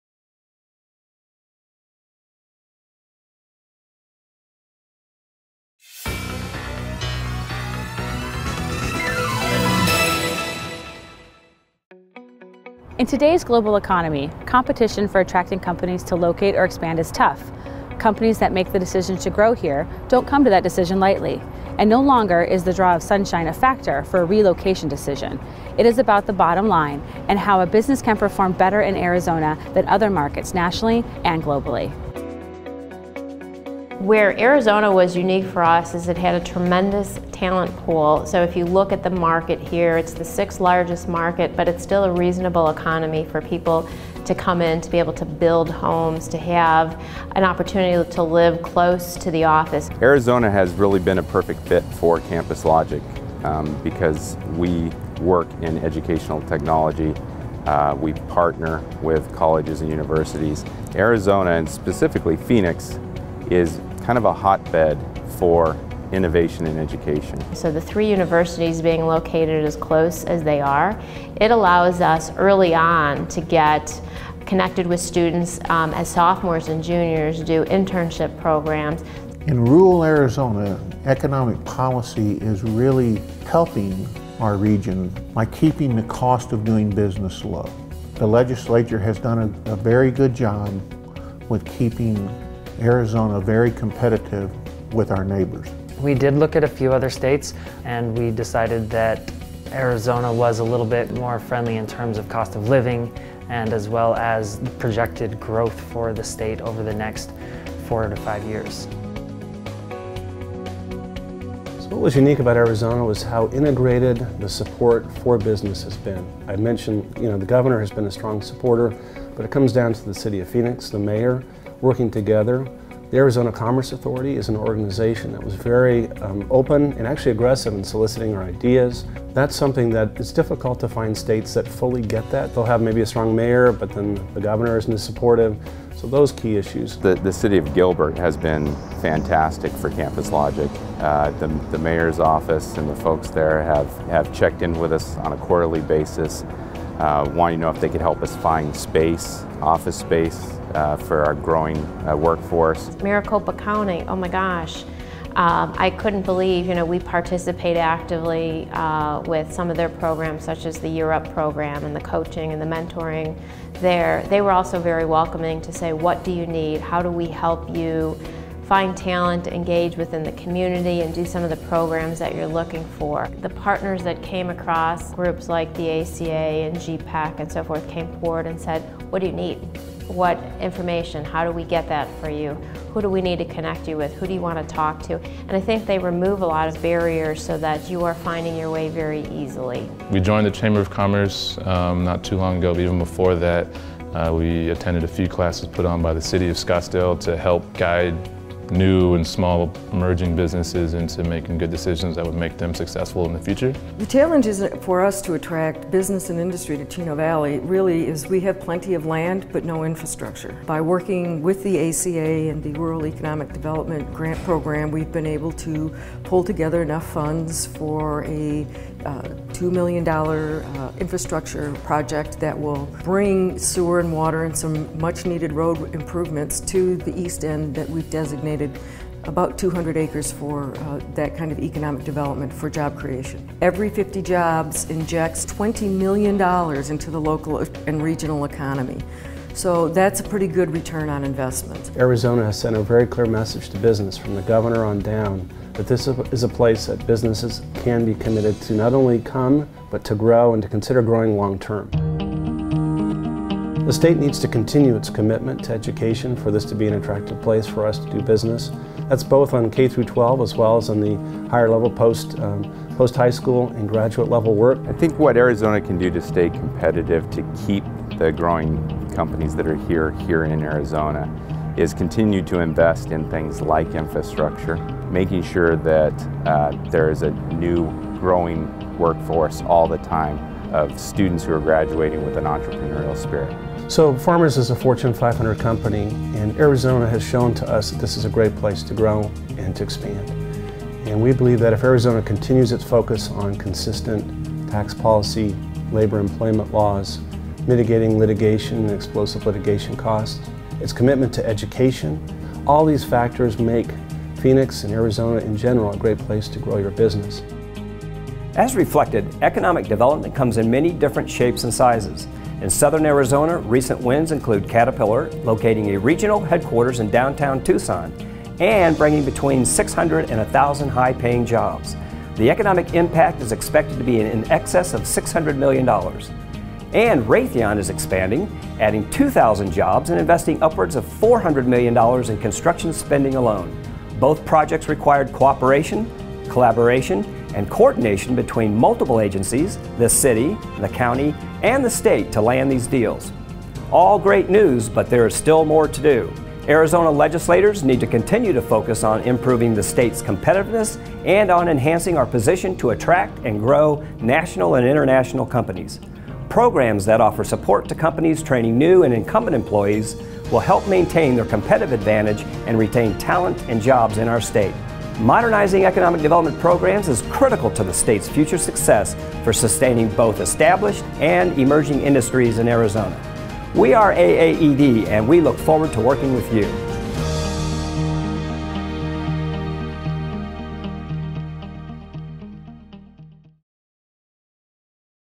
Policy Matters
AAED premiered the video “Policy Matters” at the Policy Impact Dinner on January 17th at the Arizona Science Center.  This video showcases the importance of policy in business location and business expansion in Arizona.  Additionally, it highlights the legislation and good economic development policy that makes a difference in Arizona and influences the jobs that are created in this state.  A big thank you to APS who collaborated with AAED to produce this outstanding video.